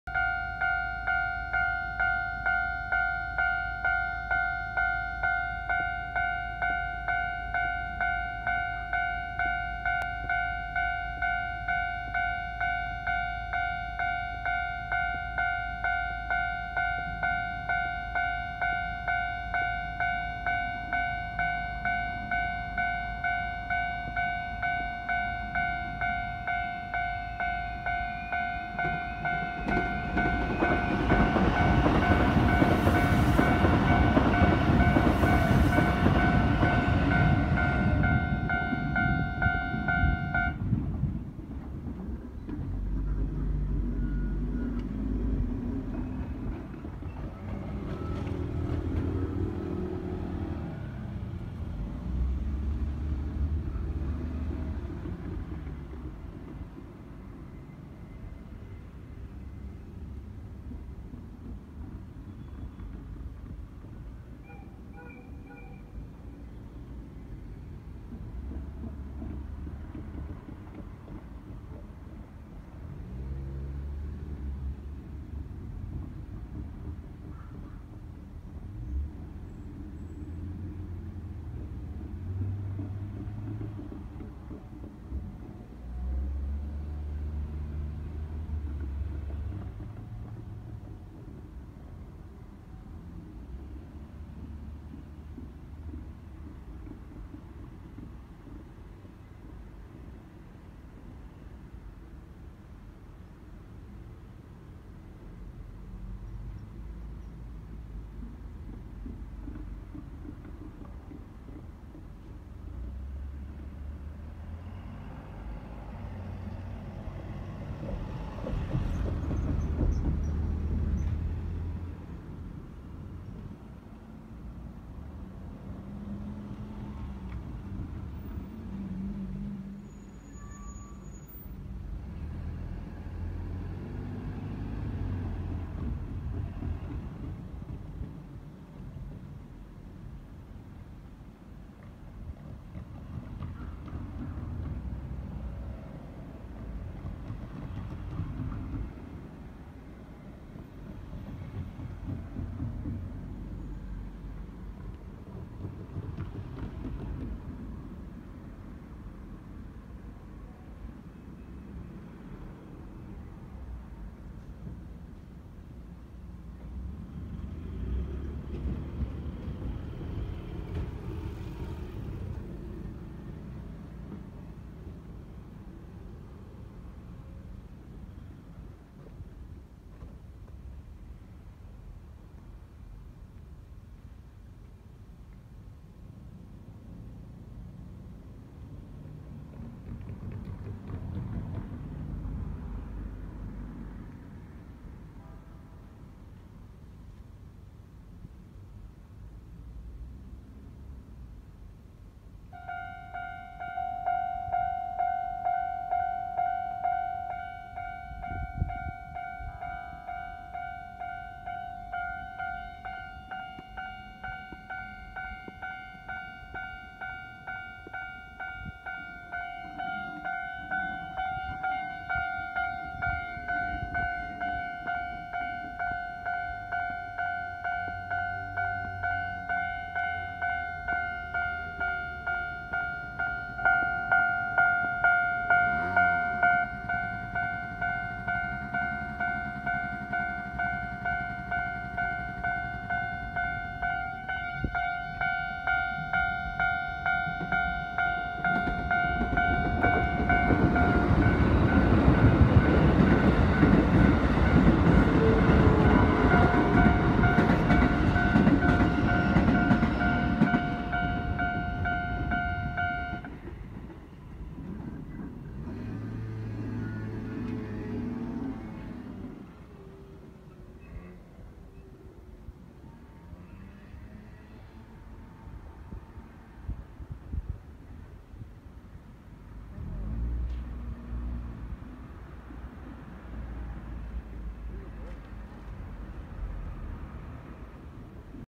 入換中のDD5515+都営新宿線1両10-300形と京急線75SH快特三崎口行き1000形217が走行！99Sエアポート急行羽田空港↔︎京急蒲田行き2100形09が停車中！【令和4年1月7日金曜日】
京急線金沢八景駅〜金沢文庫駅間の金沢文庫第2踏切から撮影。

※サムネイルの写真の撮影者も猫ぷに。(令和4年1月7日金曜日15時03分撮影。株式会社総合車両製作所(J-TREC)で入換作業中の、DD5515ディーゼル機関車+出場前の都営新宿線1両10-300形。金沢文庫駅〜金沢八景駅間を走行中の、京急線下り8両編成75SH快特三崎口行き1000形217〜224編成。)

令和4年1月7日金曜日15時03分撮影。

株式会社総合車両製作所(J-TREC)に停車・入換中の、
D-502+出場前のマニラ南北線新型車両6両編成(停車中)

DD5515ディーゼル機関車+出場前の都営新宿線1両10-300形(入換作業中)
の車両。

金沢八景駅〜金沢文庫駅間の金沢文庫第2踏切を走行中の、
京急線上り普通金沢文庫行き1000形ステンレス車両

京急線下り8両編成75SH快特三崎口行き1000形217〜224編成

京急線下り6両編成19Dエアポート急行逗子・葉山行き1500形1589、1592編成

京急線上り8両編成快特品川方面泉岳寺行き2100形32〜25編成けいきゅん
の京急線の電車。

金沢文庫検車区の車止めに停車中の、
京急線6両編成49回送1000形613〜618編成

京急線8両編成99Sエアポート急行羽田空港↔︎京急蒲田行き2100形09〜16編成けいきゅん

京急線4両編成51D回送1000形445〜448編成
の京急線の電車。

撮影後、バッテリー切れで次の入換作業の撮影は不可能と判断してこの日の撮影を終了。